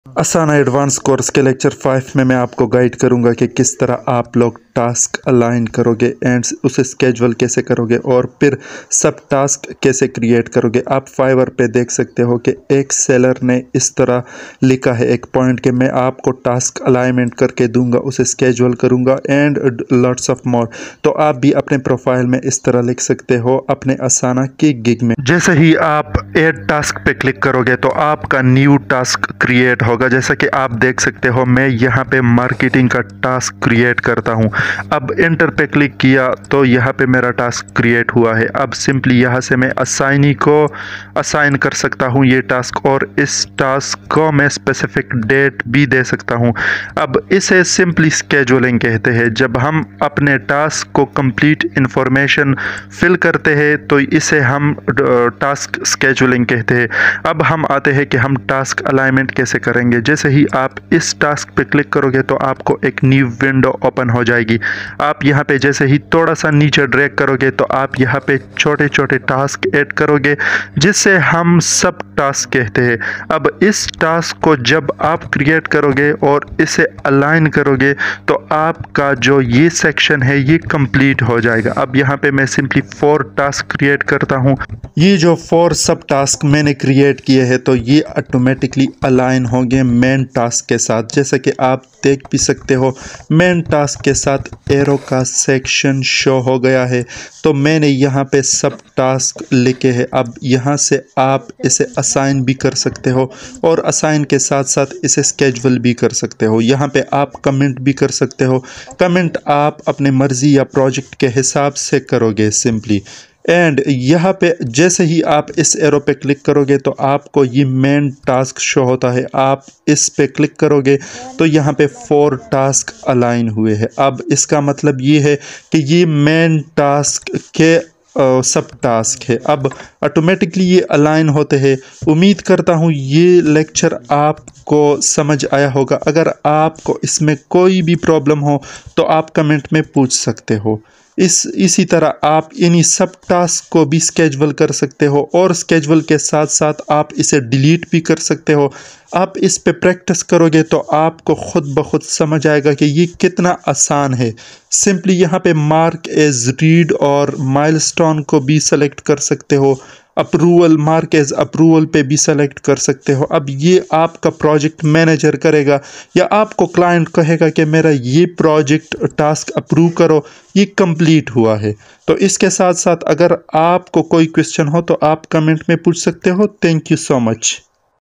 Lecture 5 - Task Alignment in Asana | Asana Advance Course 2023
Task alignment in Asana refers to the process of ensuring that all tasks are connected to a larger goal or project. This helps to ensure that everyone involved in the project is working towards the same objectives, and that each task is contributing to the overall success of the project.

To align tasks in Asana, you can follow these steps:

Define the project goals: Start by clearly defining the goals and objectives of the project. This will help you determine which tasks are necessary to achieve those goals.

Break down the project into smaller tasks: Divide the project into smaller, more manageable tasks. This will help you keep track of what needs to be done and who is responsible for each task.

Assign tasks to team members: Assign each task to the team member who is best suited to complete it.

Establish deadlines: Set deadlines for each task to ensure that they are completed on time.

Use task dependencies: Use task dependencies to ensure that tasks are completed in the correct order. For example, you may need to complete one task before another can begin.

Monitor progress: Use Asana's progress tracking tools to monitor the status of each task and ensure that everything is on track.

By aligning tasks in Asana, you can help ensure that everyone is working towards the same objectives, and that each task is contributing to the overall success of the project.